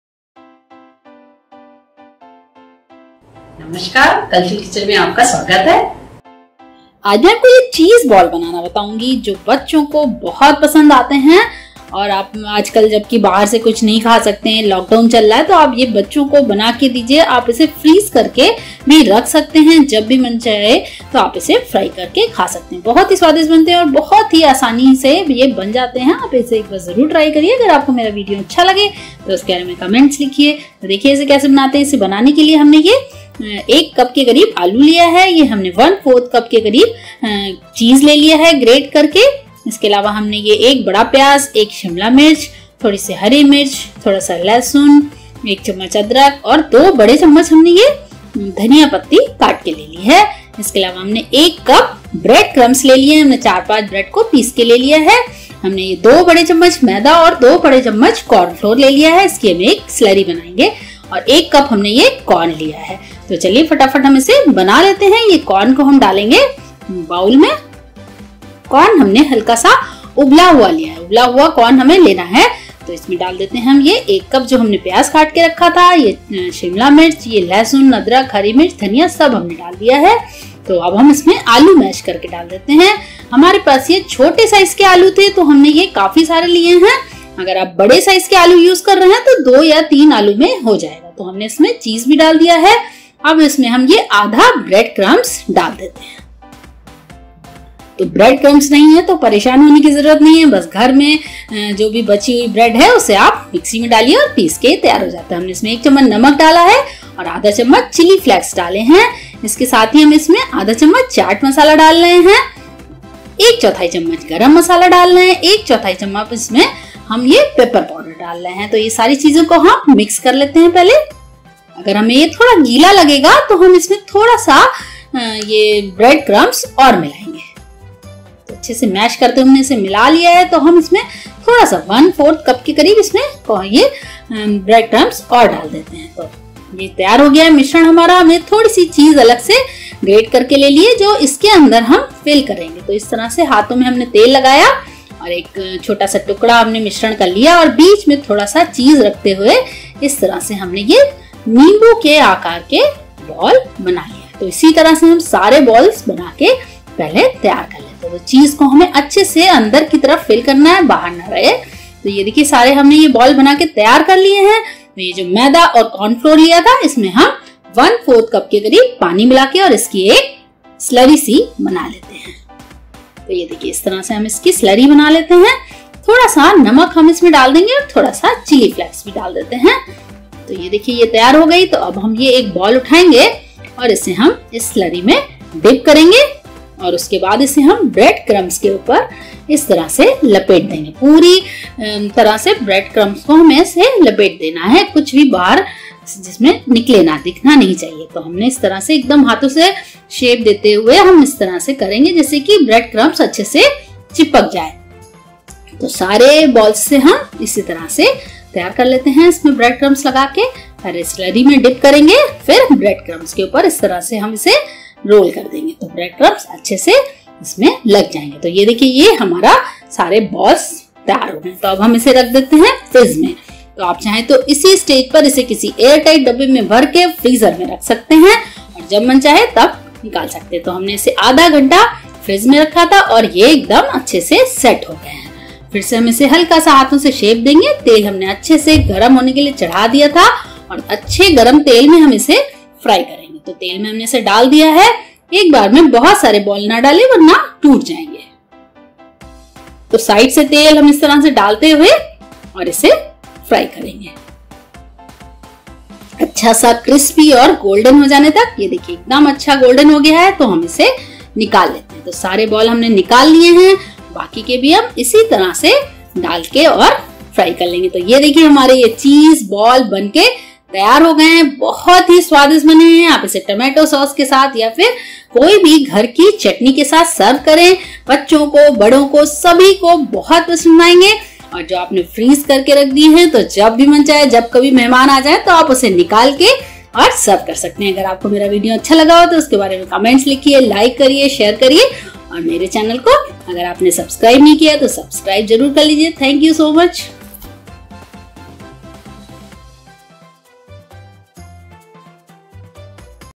Crispy corn cheese Balls | Potato cheese Balls | Kids Favorite Recipe | Lockdown Recipe
Ingredients
1 cup boiled corn
1 cup boiled potatoes
1 cup bread crumps
1/4 cup grated cheese
1 small capsicum
1 small onion
1 tsp garlic
1 tsp ginger
1 green chilli
2 tbsp coriander
1 tsp chilli flacks
1/2 tsp cumin powder
1/4 tsp pepper powder
1/4 tsp garam masala
1 tsp salt
2 tbsp maida
2 tbsp corn starch